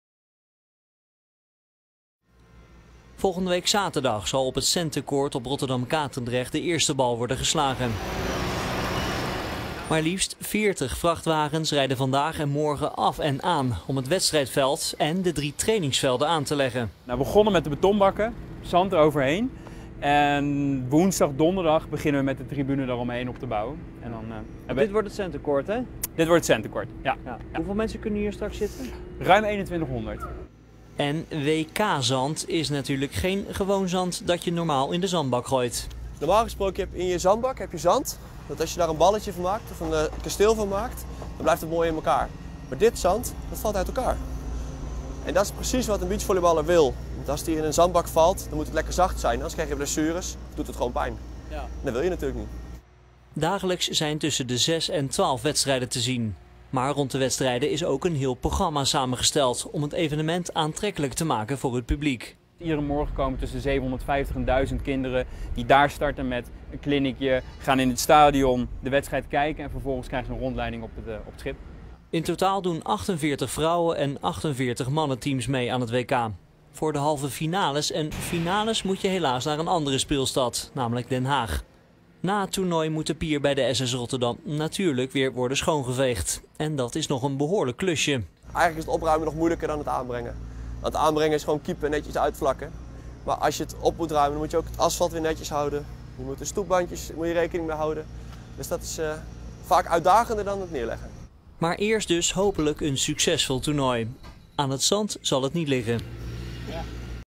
SS Rotterdam krijgt zandbakken voor de deur
Vrachtwagens vol zand rijden maandag af en aan op Katendrecht. Ze storten zand voor het WK Beachvolleybal, dat vanaf 26 juni in Nederland plaatsvindt. Rotterdam is een van de speelsteden. Met een prachtig decor: het SS Rotterdam.

   Lees meer: SS Rotterdam krijgt zandbakken voor de deur